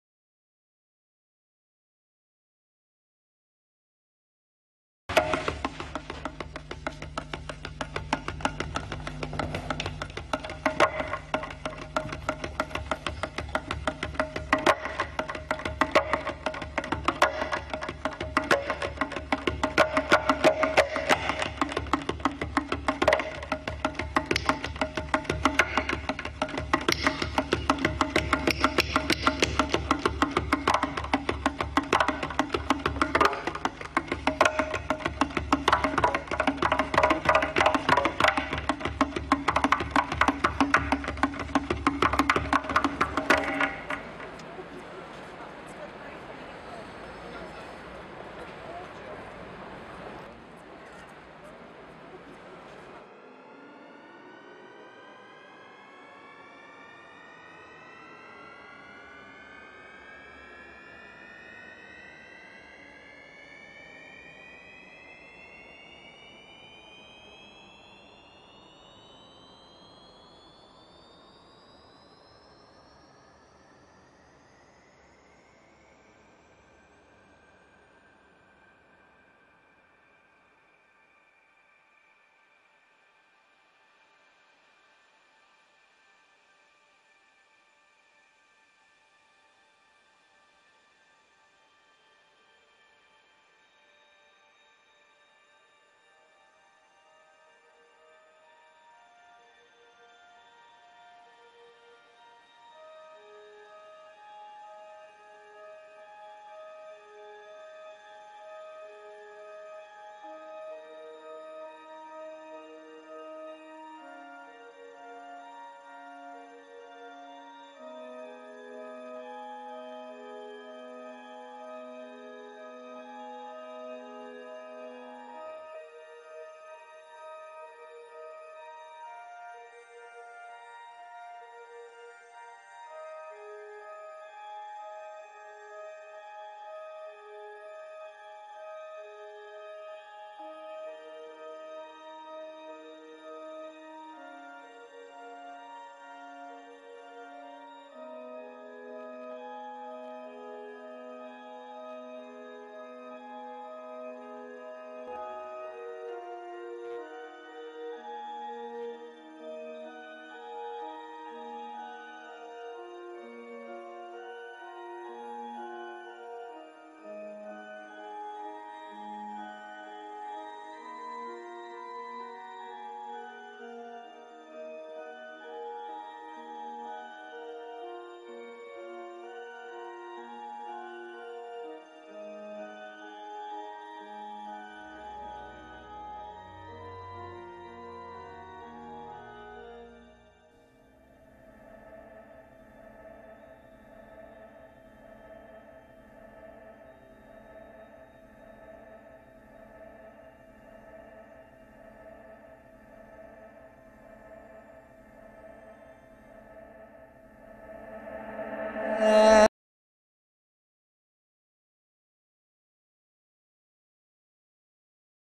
Us and Them (Short Film)
A short film by Sam Karamyani shot around Piccadilly Circus, London. Please comment and tell me your thoughts on this video. Watch in HD and please rate and comment. Thanks.